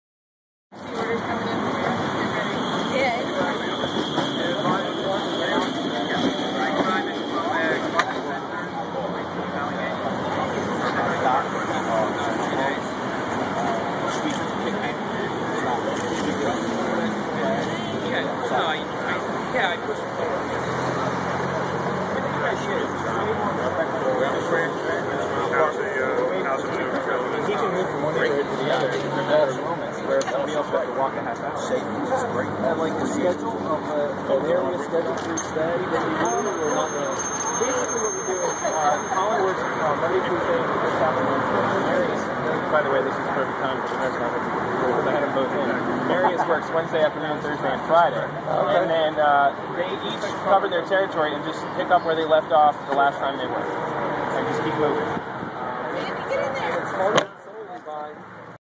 Trash-Collecting Tricycle Hits the Streets of Kensington's Commercial Corridor - 1
Philadelphia Mayor Michael Nutter came from across the street, where he was holding a press conference addressing blight reform, to see what the fuss was about and took the trike for a brief spin.
DroidX video by Rikard Larma